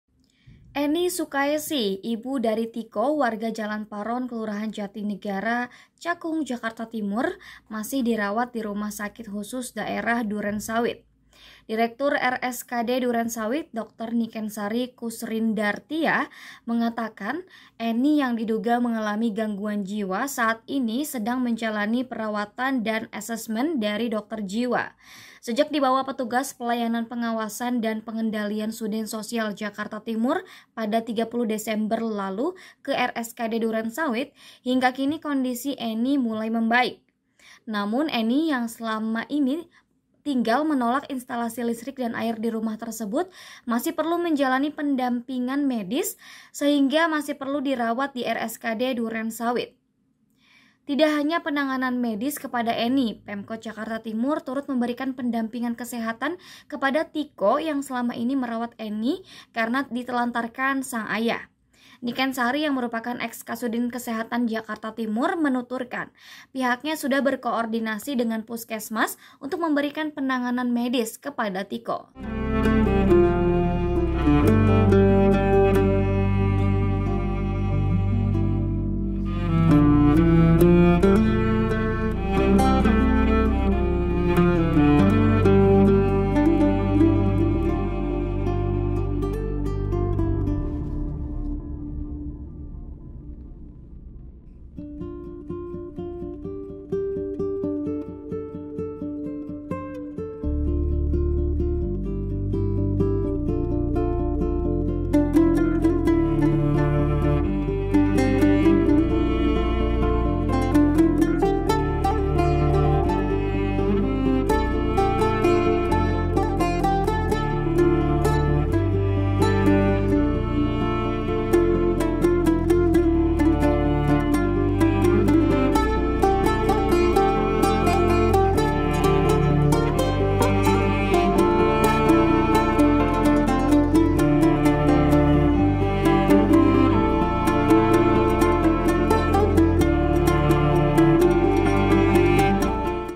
Ibu Eny yang Diduga Alami Gangguan Kejiwaan Dirawat di RSKD Duren Sawit, Dokter Beberkan Kondisinya
Laporan Wartawan TribunJakarta.com, Bima Putra

TRIBUN-VIDEO.COM - Eny Sukaesi, ibu dari Tiko warga Jalan Paron, Kelurahan Jatinegara, Kecamatan Cakung, Jakarta Timur masih dirawat di Rumah Sakit Khusus Daerah (RSKD) Duren Sawit.

Direktur RSKD Duren Sawit, dr. Nikensari Koesrindartia mengatakan Eny yang diduga mengalami gangguan jiwa saat ini sedang menjalani perawatan dan assement dari dokter jiwa.

“Dilayani dengan baik dikonsulkan dengan dokter ahli jiwa. Saat ini dalam penanganan medis dan pendampingan oleh dokter psikiater," kata Nikensari dalam keterangannya, Kamis (5/1/2023).

Sejak dibawa petugas pelayanan pengawasan dan pengendalian (P3S) Sudin Sosial Jakarta Timur pada 30 Desember 2022 lalu ke RSKD Duren Sawit, hingga kini kondisi Eny membaik.

Namun Eny yang selama tinggal menolak instalasi listrik dan air di rumah tersebut masih perlu menjalani pendampingan medis, sehingga masih perlu dirawat di RSKD Duren Sawit.

"Kondisi terakhir beliau sudah lebih baik meski demikian masih perlu pendampingan agar Ibu Eny dapat lebih terkontrol lagi,” ujarnya.

Tidak hanya penanganan medis kepada Eny, Pemkot Jakarta Timur turut memberikan pendampingan kesehatan kepada Tiko yang selama ini merawat Eny karena ditelantarkan sang ayah.

Nikensari yang merupakan eks Kasudin Kesehatan Jakarta Timur menuturkan pihaknya sudah berkoordinasi dengan Puskesmas untuk memberikan penanganan medis kepada Tiko.

“Kami sudah berkoordinasi dengan Puskesmas Kecamatan Cakung untuk mengatur waktunya,” tuturnya.

Untuk penanganan jangka panjang kepada Eny, Pemkot Jakarta Timur menawarkan kepada Tiko agar sang ibu dapat ditangani di Panti Werdha yang berada di bawah naungan Dinas Sosial DKI Jakarta.

Kepala Sudin Sosial Jakarta Timur, Purwono menuturkan bila Tiko mengizinkan pihaknya siap memfasilitasi agar Eny dirujuk ke Panti Werdha yang berada di Kecamatan Cipayung.

“Kami siapkan semua, gratis. Anaknya boleh datang berkunjung setiap hari. Enggak ada masalah,” tutur Purwono.

Artikel ini telah tayang di TribunJakarta.com dengan judul Dokter Ahli Jiwa RSKD Duren Sawit Tangani Perawatan Ibunda Tiko, Begini Kondisi Terakhir Ibu Eny,
Penulis: Bima Putra | Editor: Ferdinand Waskita Suryacahya